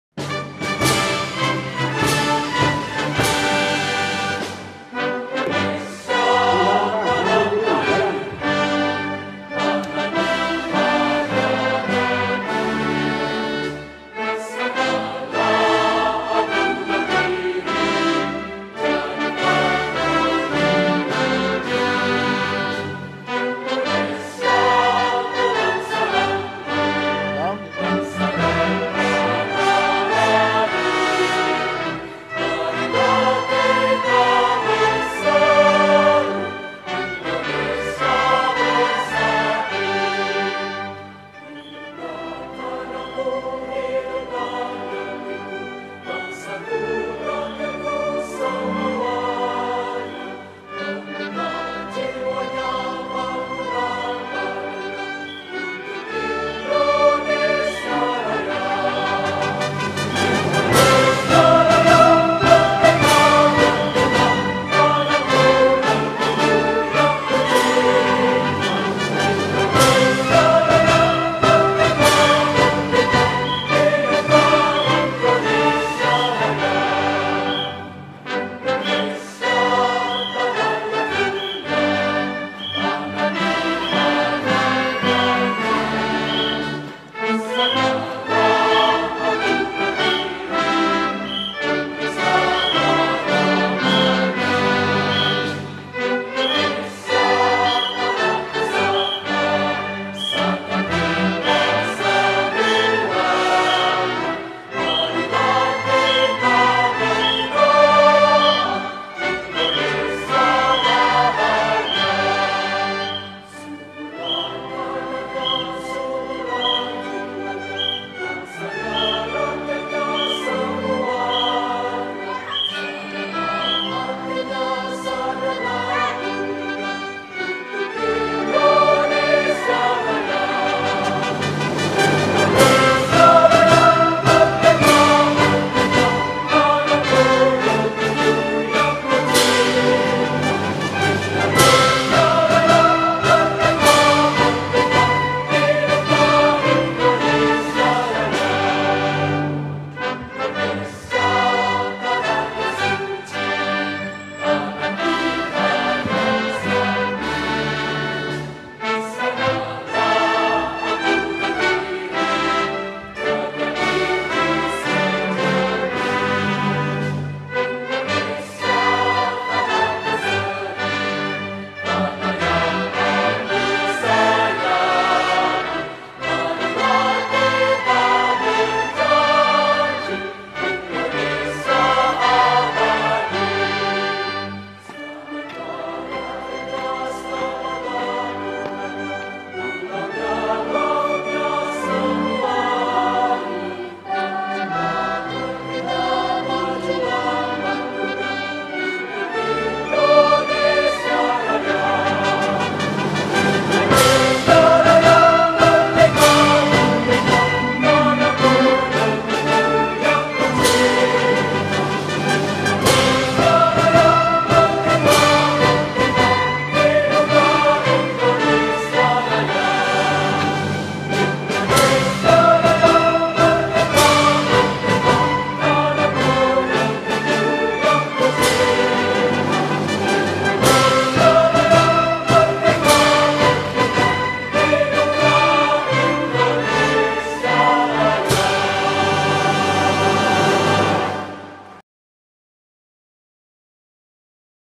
Upacara pembukaan (mos) tahun 2017 MTs Ma'arif NU 11 Purbasari
pemukaan masa orientasi siswa baru dibuka denganu upacara pembukaan yang di iringi dengan pasukan paskibra